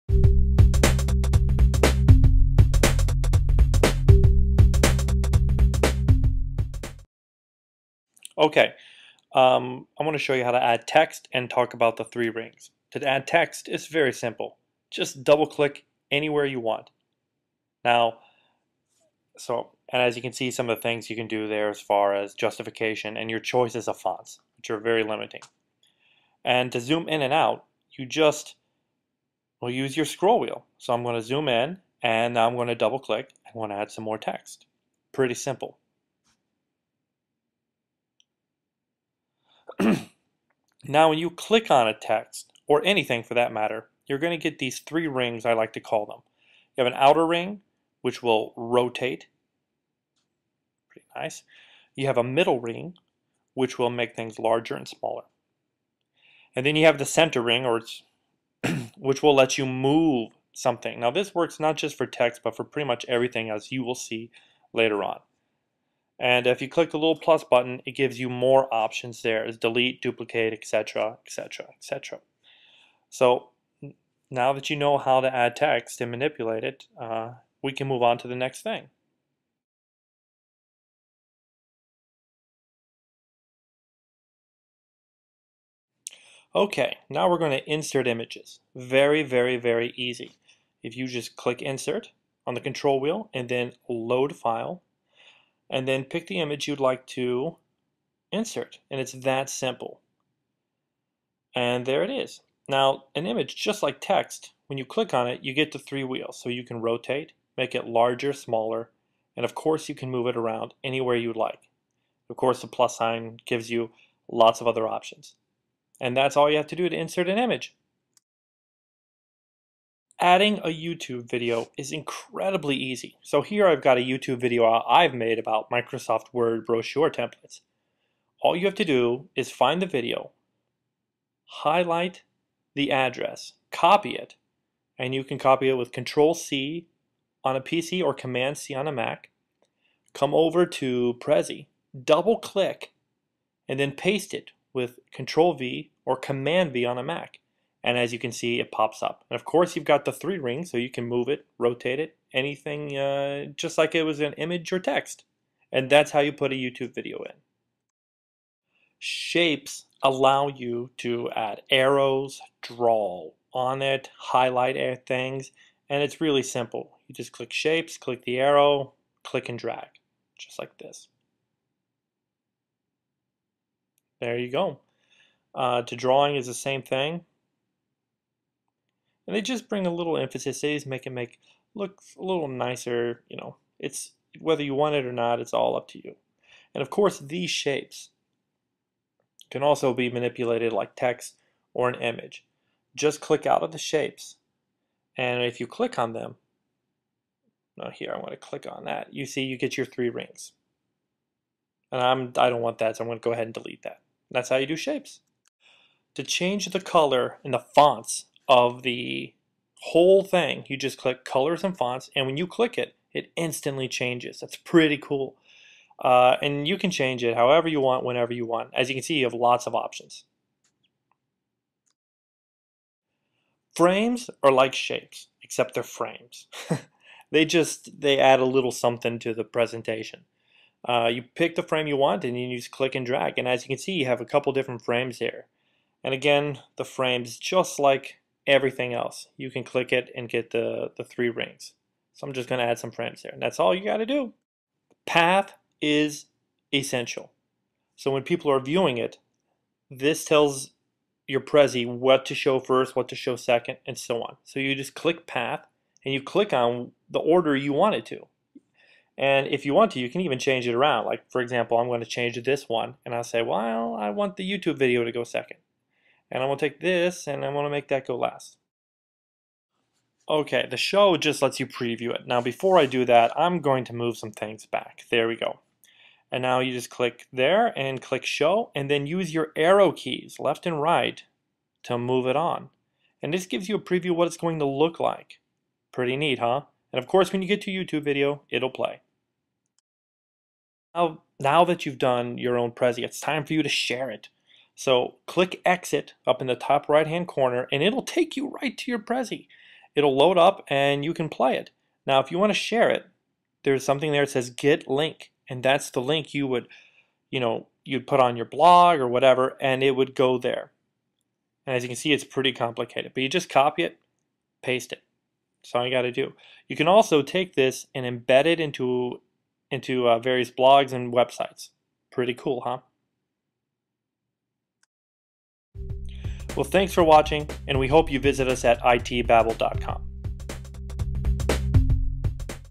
Prezi-How to do it
This is a quick video to show you how to do the basics of Prezi.  It will cover how to add text, images, YouTube videos, frames, shapes, how to create the path, and how to share it.  Prezi makes it easy, but it is always nice to have a video to help.